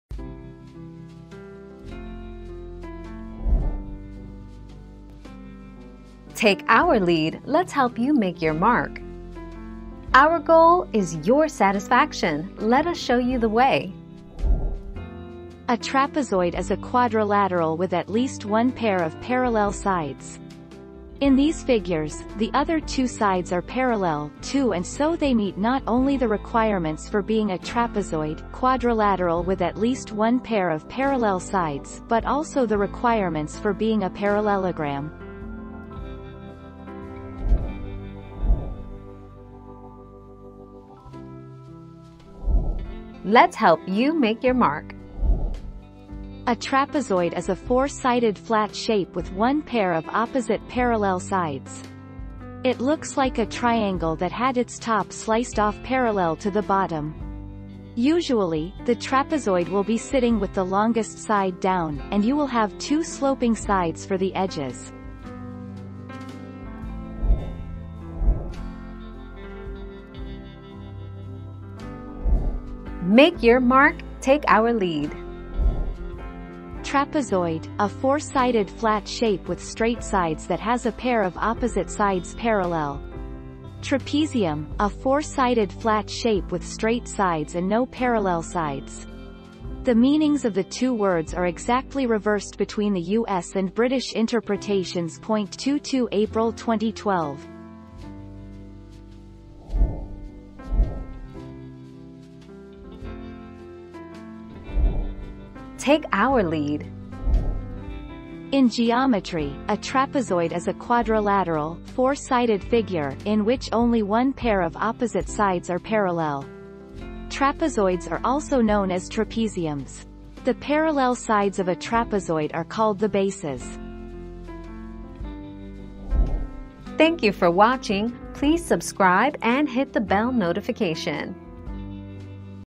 What makes a shape a trapezoid?
00:00 - What makes a shape a trapezoid?
00:39 - What is a trapezoid shape look like?
01:09 - What is the difference between a trapezium and a trapezoid?
01:41 - How do you tell if a shape is a trapezoid?

Laura S. Harris (2021, March 2.) What makes a shape a trapezoid?
    AskAbout.video/articles/What-makes-a-shape-a-trapezoid-223876